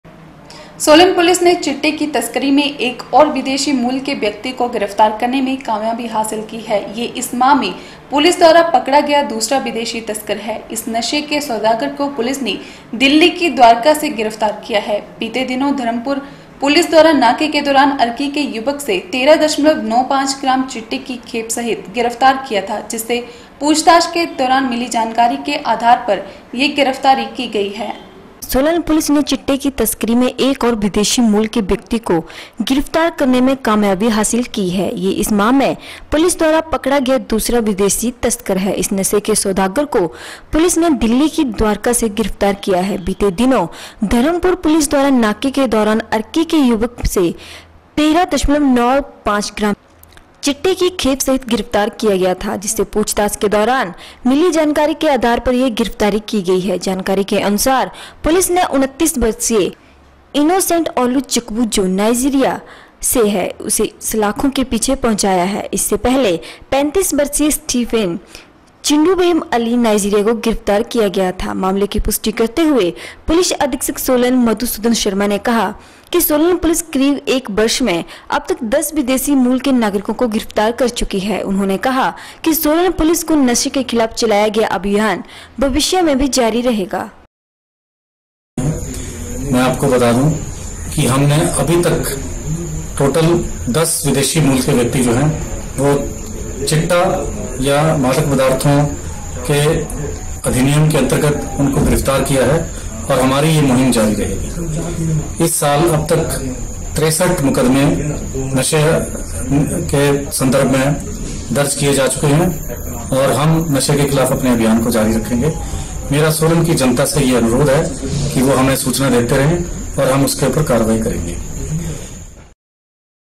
7 SEP N 16 Succeeded in arresting another person of foreign origin in Chitta smuggling
सोलन पुलिस ने चिट्टे की तस्करी में एक और विदेशी मूल के व्यक्ति को गिरफ्तार करने में कामयाबी हासिल की है | यह इस माह में पुलिस द्वारा पकड़ा गया दूसरा विदेशी तस्कर है  | इस नशे के सौदागर को पुलिस ने  दिल्ली की द्वारका  से गिरफ्तार किया है। बीते दिनों धर्मपुर पुलिस द्वारा नाके के दौरान अर्की के युवक से 13. 95 ग्राम चिट्टे की खेप सहित गिरफ्तार किया था। जिससे पूछताछ के दौरान मिली जानकारी आधार पर यह गिरफ्तारी की गई है।